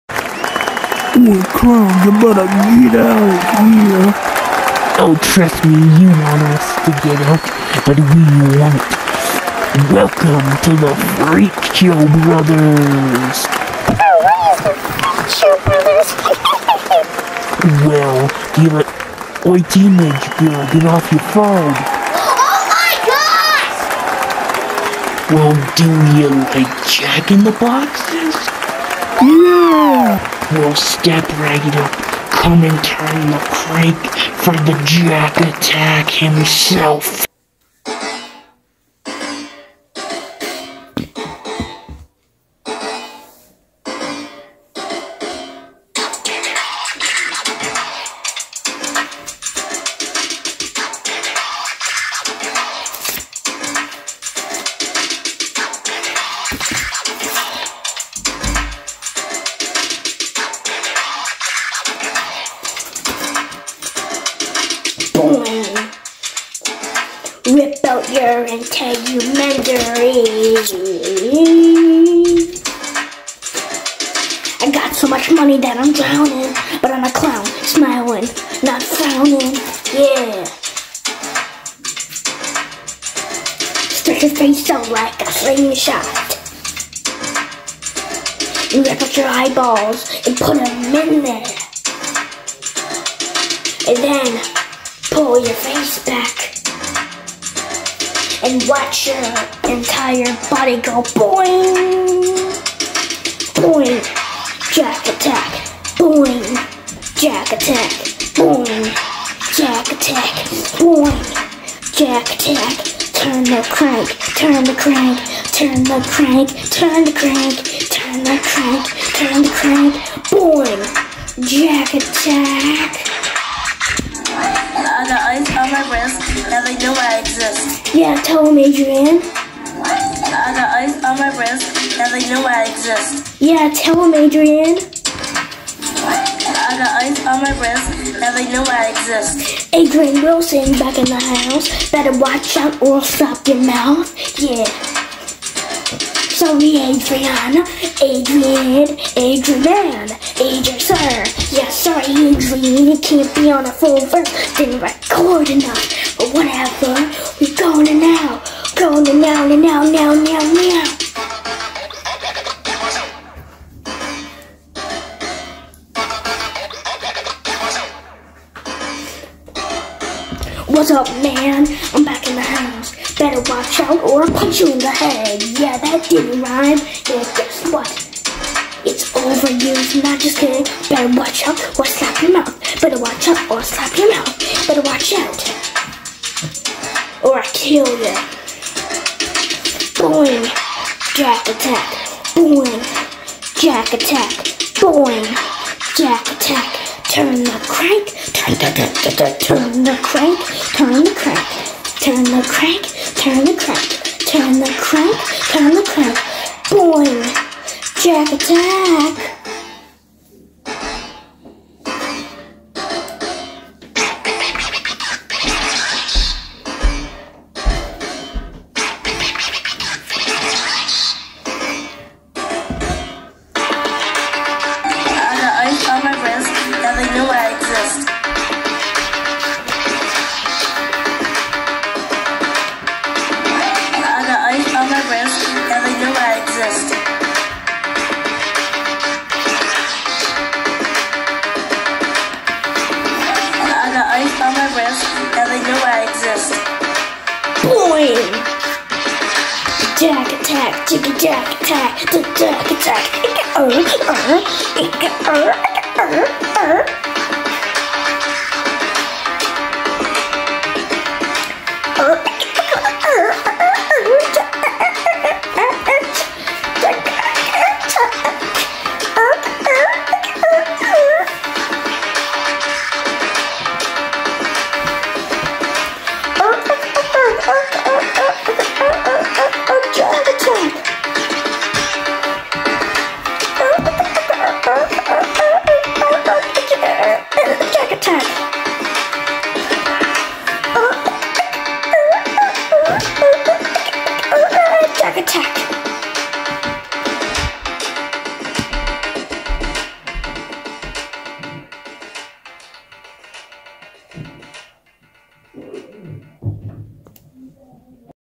The Freakshow Brothers: Sampler
As you walk through the exhibit of carnival frights.. the brothers could take you on a scare spike.. but the brothers will kill.. they are simply known as.. Kevin and Kent.